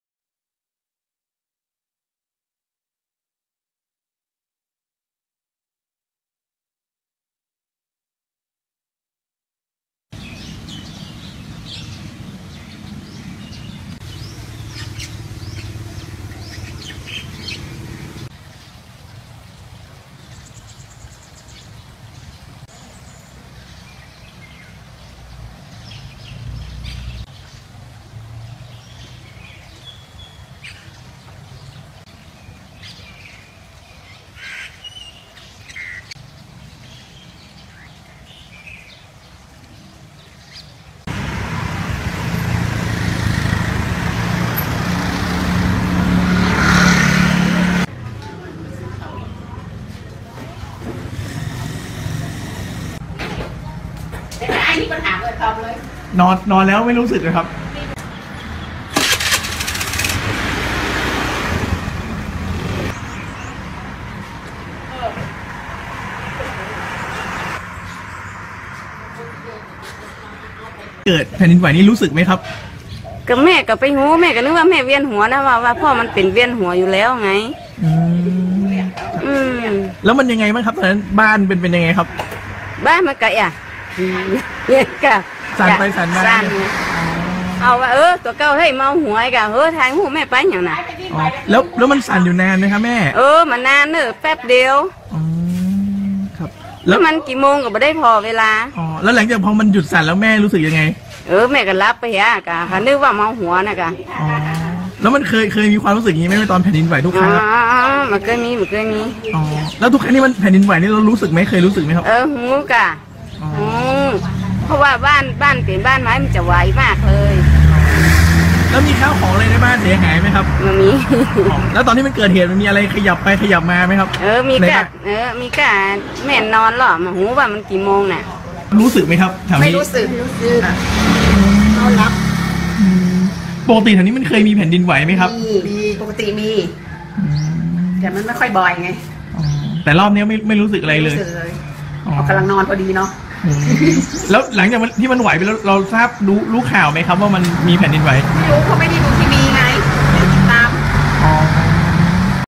ชาวบ้านสันทรายเผยนาทีแผ่นดินไหว3.2ริกเตอร์เห็นบ้านโอนเอนแต่นึกว่าเวียนหัว
23/01/2019 ชาวบ้านสันทรายเผยนาทีแผ่นดินไหว3.2ริกเตอร์เห็นบ้านโอนเอนแต่นึกว่าเวียนหัว
ศูนย์ข่าวเชียงใหม่-แผ่นดินไหว 3.2 ตามมาตราริกเตอร์ ที่สันทราย จังหวัดเชียงใหม่ แรงสั่นสะเทือนรู้สึกได้หลายจุด แต่ไม่มีรายงานความเสียหาย ชาวบ้านเผยช่วงเกิดเหตุเหมือนบ้านโอนเอนได้แต่เข้าใจว่าป่วยจนเวียนหัว